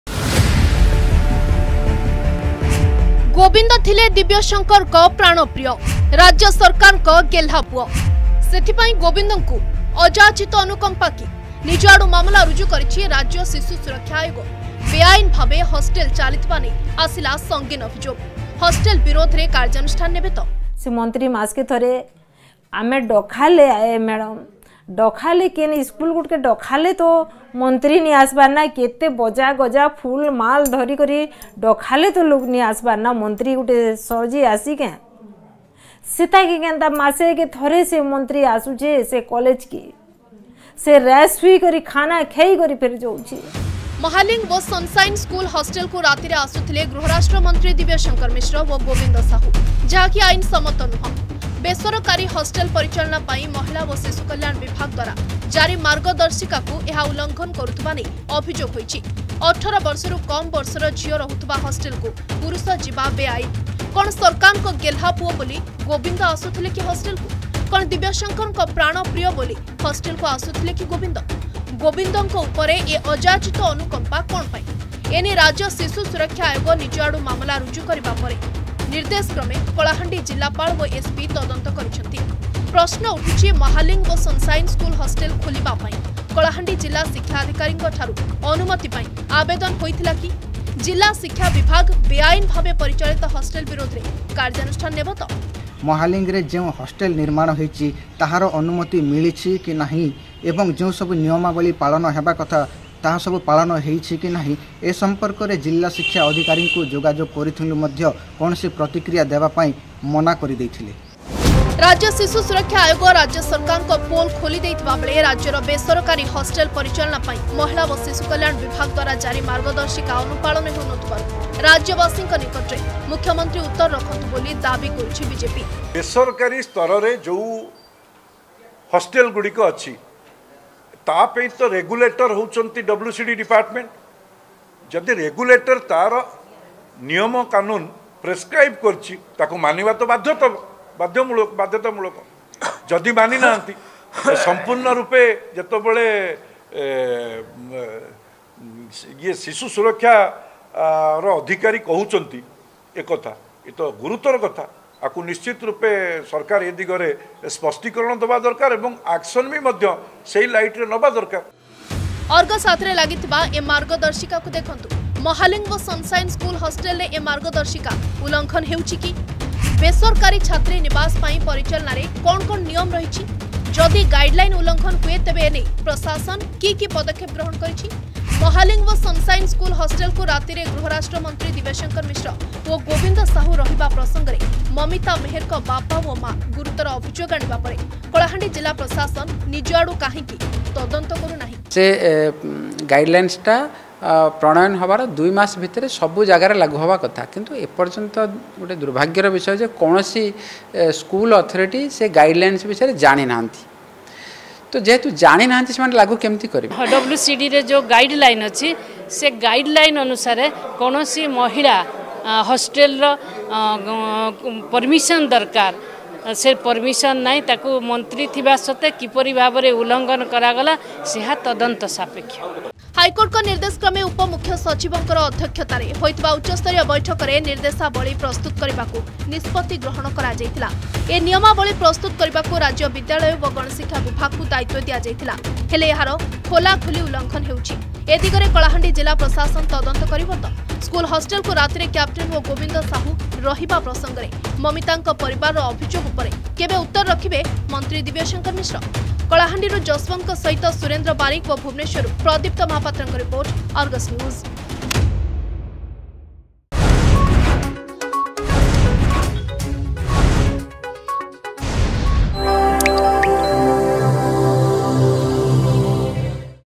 ବେଆଇନ ଭାବେ ହଷ୍ଟେଲ ଚାଲିଥିବା ନେଇ ଆସିଲା ସଙ୍ଗୀନ ଅଭିଯୋଗ
ଗୋବିନ୍ଦ ଥିଲେ ଦିବ୍ୟଶଙ୍କରଙ୍କ ପ୍ରାଣ ପ୍ରିୟ । ରାଜ୍ୟ ସରକାରଙ୍କ ଗେହ୍ଲାପୁଅ । ସେଥିପାଇଁ ଗୋବିନ୍ଦକୁ ଅଯାଚିତ ଅନୁକମ୍ପା କି? । ସରକାରଙ୍କ ନିୟମ କାନୁନ ଭାଙ୍ଗି ବେଆଇନ ଭାବେ ହଷ୍ଟେଲ ଚାଲିଥିବା ନେଇ ସଙ୍ଗୀନ ଅଭିଯୋଗ । ଆଉ ଏଥିପାଇଁ ନିଜ ଆଡୁ ମାମଲା ରୁଜୁ କରିଛି ରାଜ୍ୟ ଶିଶୁ ସୁରକ୍ଷା ଆୟୋଗ । ବେଆଇନ ହଷ୍ଟେଲ ବିରୋଧରେ କାର୍ଯ୍ୟାନୁଷ୍ଠାନ ନେବେ ତ ?